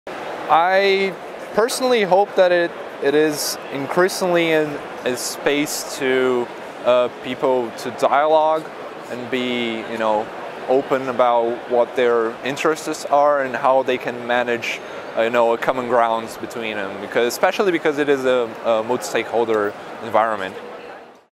Global IGF 2015: Rodrigo Moura Karolczak on Hope for IGF Future
Rodrigo Moura Karolczak, a researcher for Escola de Direito de São Paulo, shares his answer to the question, “What is your greatest hope for the future of the Global Internet Governance Forum – What should it accomplish for the world?”Recorded at the Global Internet Governance Forum 2015 in João Pessoa, Brazil, by the Imagining the Internet Center, an initiative of Elon University, North Carolina, USA.